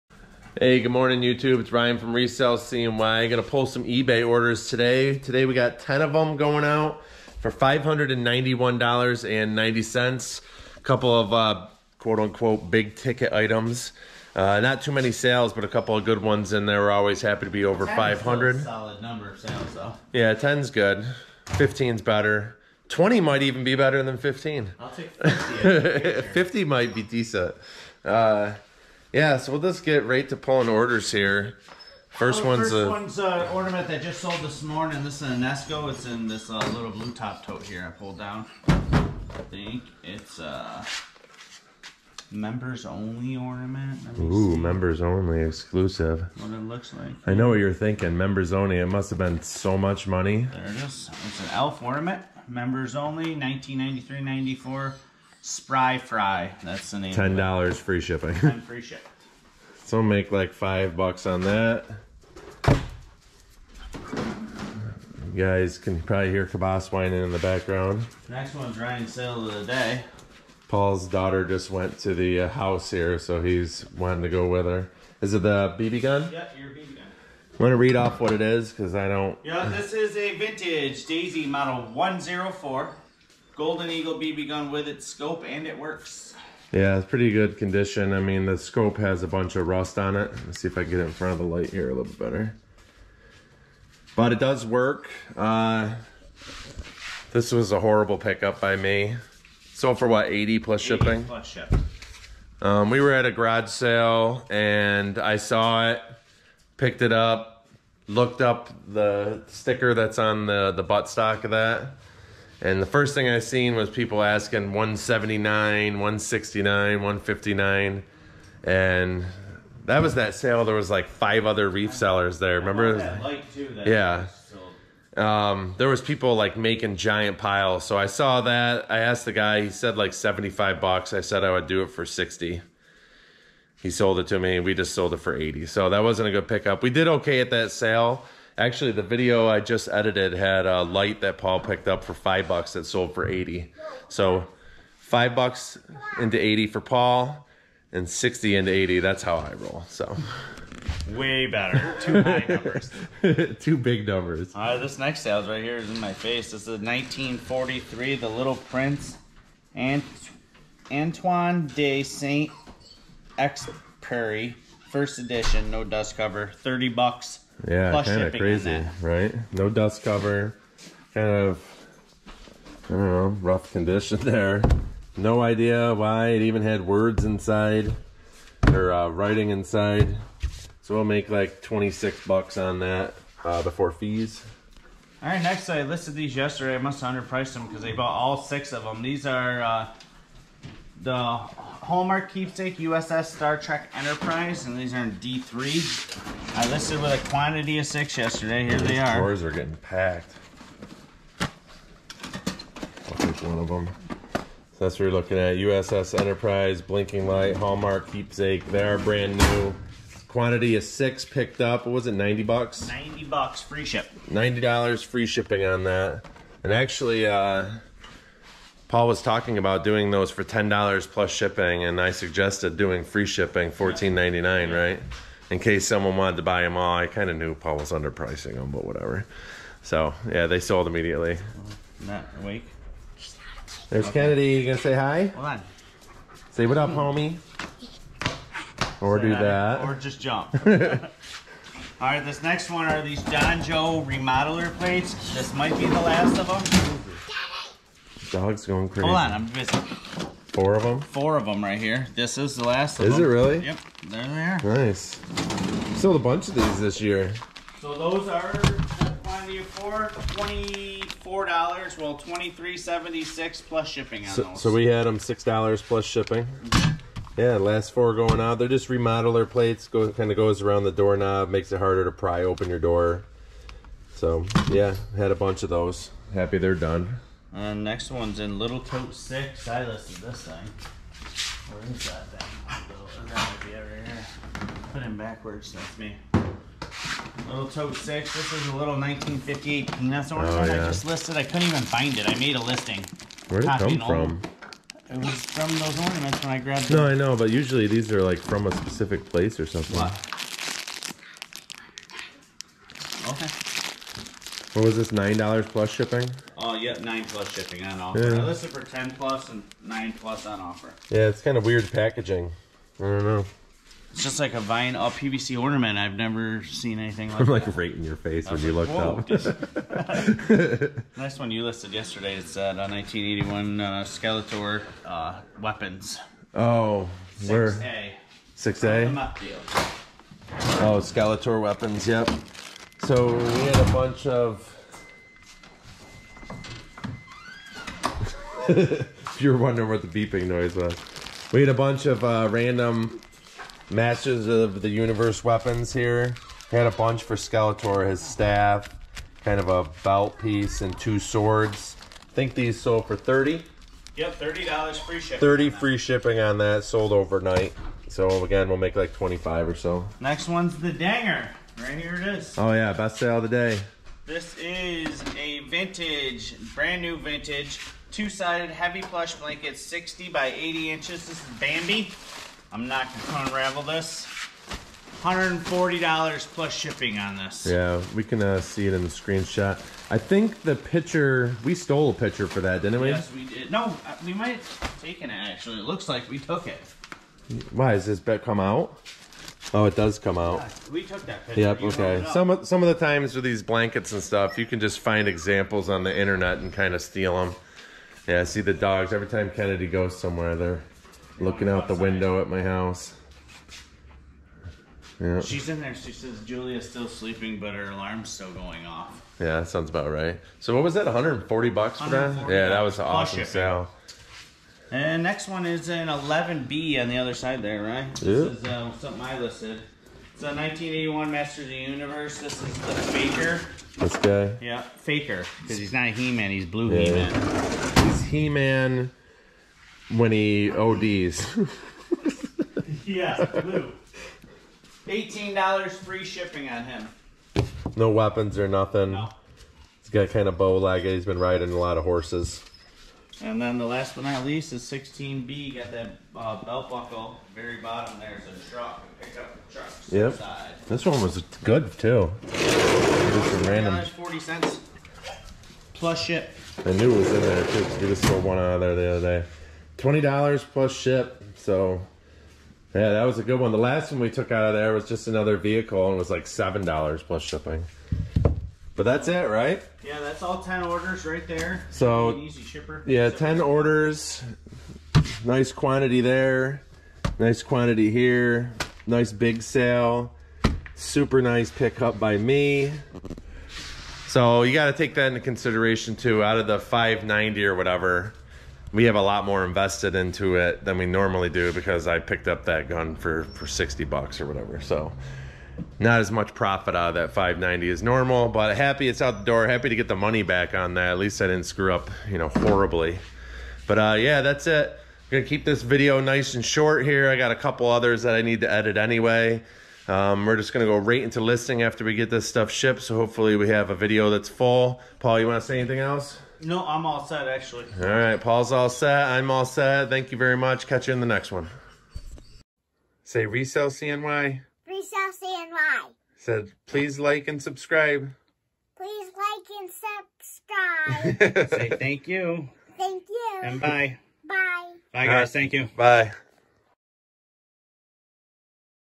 Nice Day of EBay Sales… & a ‘GREAT’ pickup lolol
Ryan and Paul here for ResellCNY… we buy things at garage, sales and yard sales and even purchase large collections to resell on eBay.
 Here is a quick ‘What sold?’video